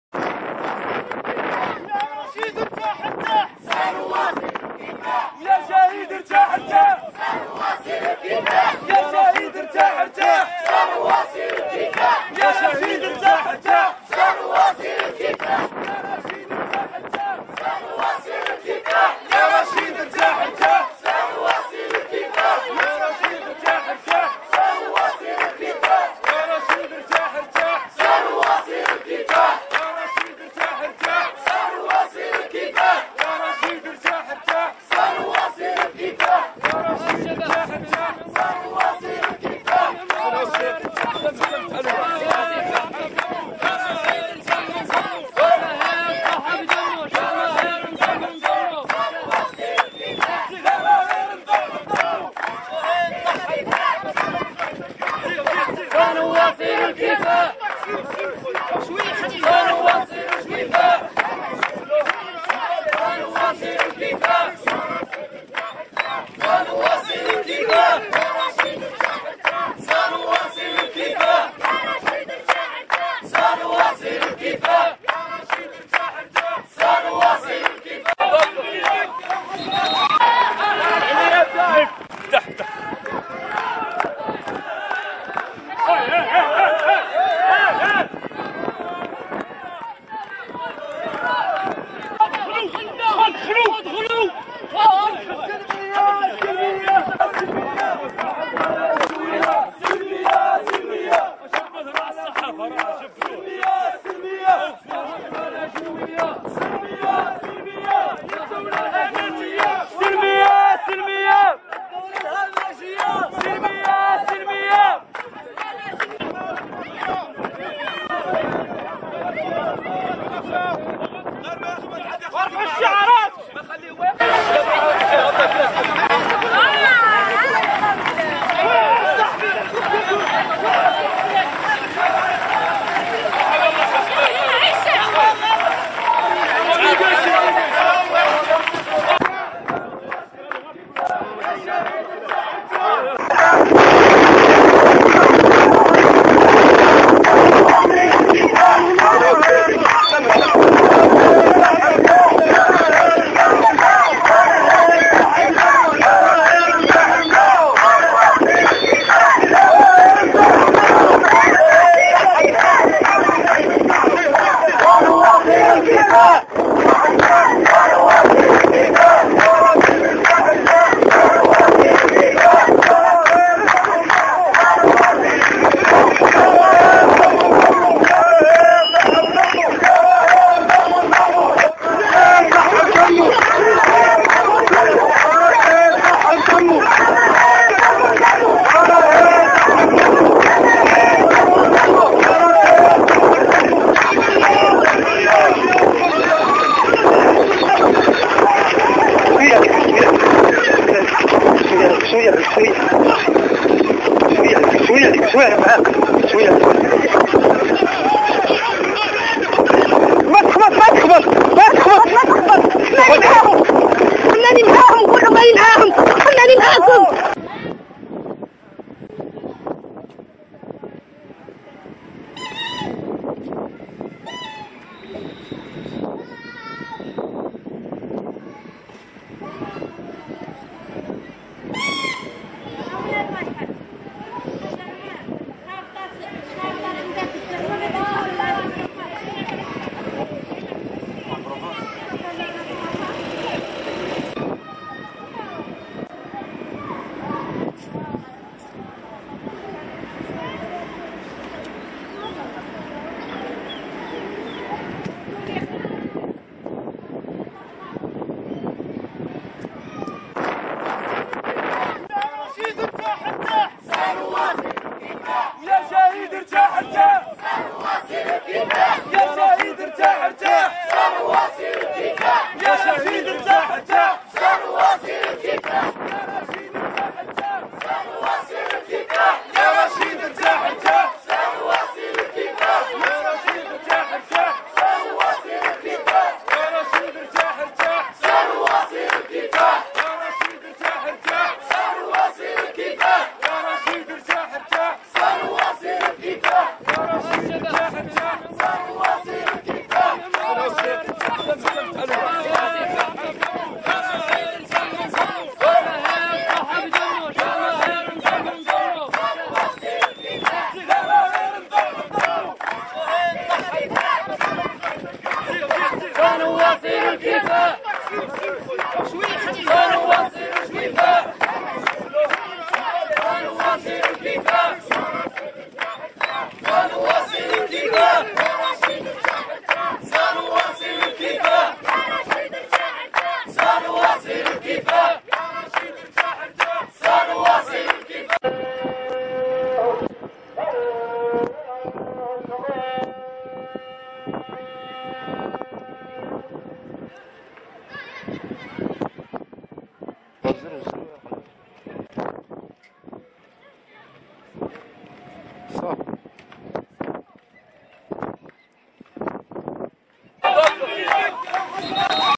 وقفة الجماهير الصحراوية تضامنا مع قلعة الصمود وتحدى 2013.09.26
وقفة الجماهير الصحراوية  بالعاصمة العيون المحتلة تضامنا مع قلعة الصمود وتحدى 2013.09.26